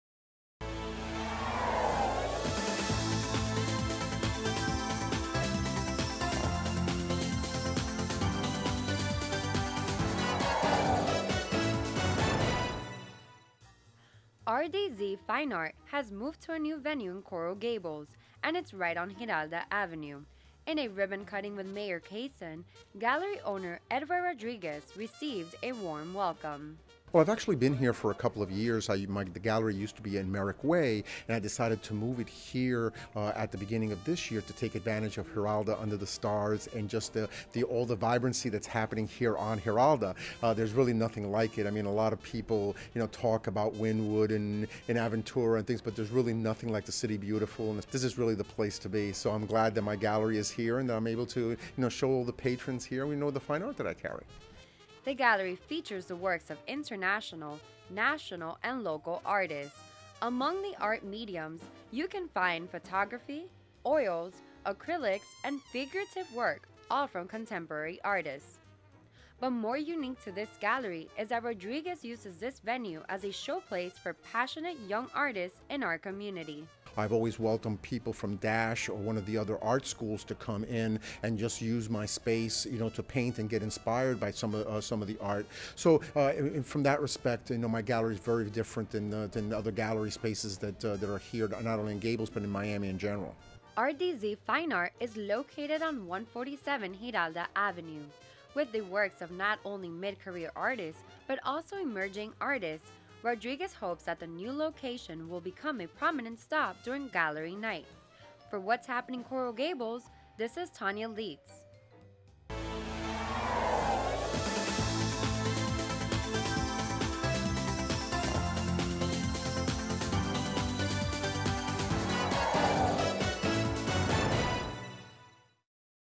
RDZ Fine Art holds its Ribbon Cutting Opening
City of Coral Gables held the formal welcoming for RDZ's move to Giralda Avenue.